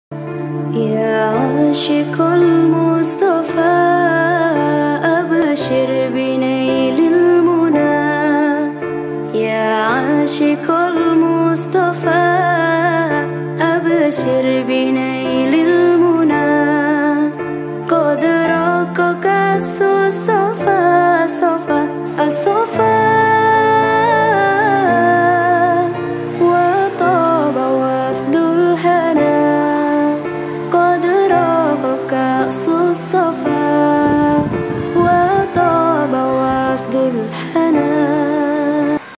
MTs N 1 Banjarnegara
Santunan yatim/piyatu siswa MTs N 1 Banjarnegara dalam memperingati Muharom